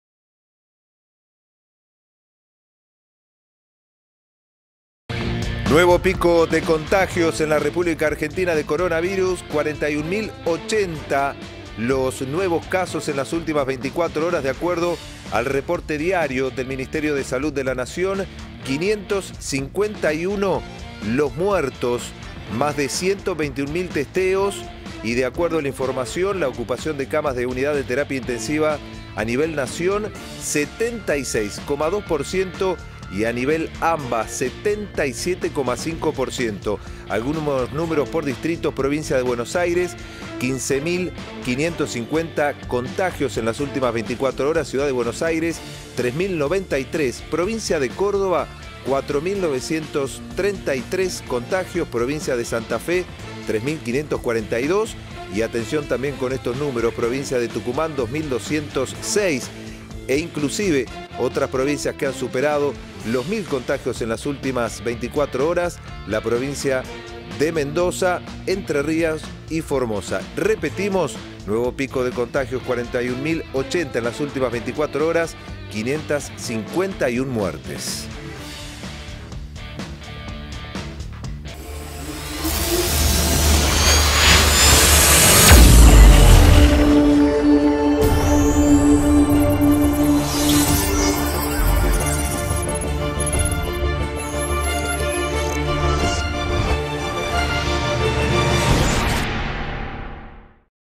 𝐑É𝐂𝐎𝐑𝐃 𝐃𝐄 𝐂𝐎𝐍𝐓𝐀𝐆𝐈𝐎𝐒 𝐃𝐄 𝐂𝐎𝐑𝐎𝐍𝐀𝐕𝐈𝐑𝐔𝐒 𝐄𝐍 𝐀𝐑𝐆𝐄𝐍𝐓𝐈𝐍𝐀, 𝐒𝐄 𝐑𝐄𝐏𝐎𝐑𝐓𝐀𝐑𝐎𝐍 𝟒𝟏.𝟎𝟖𝟎 𝐄𝐍 𝐋𝐀𝐒 Ú𝐋𝐓𝐈𝐌𝐀𝐒 𝟐𝟒 𝐇𝐎𝐑𝐀𝐒
𝐉𝐎𝐑𝐍𝐀𝐃𝐀 𝐑É𝐂𝐎𝐑𝐃 𝐃𝐄 𝐂𝐎𝐍𝐓𝐀𝐆𝐈𝐎𝐒 𝐃𝐄 𝐂𝐎𝐑𝐎𝐍𝐀𝐕𝐈𝐑𝐔𝐒 𝐄𝐍 𝐀𝐑𝐆𝐄𝐍𝐓𝐈𝐍𝐀, 𝐒𝐄 𝐑𝐄𝐏𝐎𝐑𝐓𝐀𝐑𝐎𝐍 𝟒𝟏.𝟎𝟖𝟎 𝐄𝐍 𝐋𝐀𝐒 Ú𝐋𝐓𝐈𝐌𝐀𝐒 𝟐𝟒 𝐇𝐎𝐑𝐀𝐒

𝘈𝘥𝘦𝘮á𝘴, 𝘦𝘭 𝘔𝘪𝘯𝘪𝘴𝘵𝘦𝘳𝘪𝘰 𝘥𝘦 𝘚𝘢𝘭𝘶𝘥 𝘥𝘦 𝘭𝘢 𝘕𝘢𝘤𝘪ó𝘯 𝘪𝘯𝘧𝘰𝘳𝘮ó 551 𝘮𝘶𝘦𝘳𝘵𝘦𝘴. 𝘊𝘰𝘯 𝘦𝘴𝘵𝘰𝘴 𝘥𝘢𝘵𝘰𝘴, 𝘦𝘭 𝘵𝘰𝘵𝘢𝘭 𝘥𝘦 𝘤𝘢𝘴𝘰𝘴 𝘥𝘦𝘴𝘥𝘦 𝘦𝘭 𝘪𝘯𝘪𝘤𝘪𝘰 𝘥𝘦 𝘭𝘢 𝘱𝘢𝘯𝘥𝘦𝘮𝘪𝘢 𝘢𝘴𝘤𝘪𝘦𝘯𝘥𝘦 𝘢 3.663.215 𝘺 𝘭𝘢𝘴 𝘷í𝘤𝘵𝘪𝘮𝘢𝘴 𝘧𝘢𝘵𝘢𝘭𝘦𝘴 𝘴𝘰𝘯 76.135.

MULTIMEDIOS PRENSA Del Interior. Noticias en tiempo real las 24hs, los 365 días del año.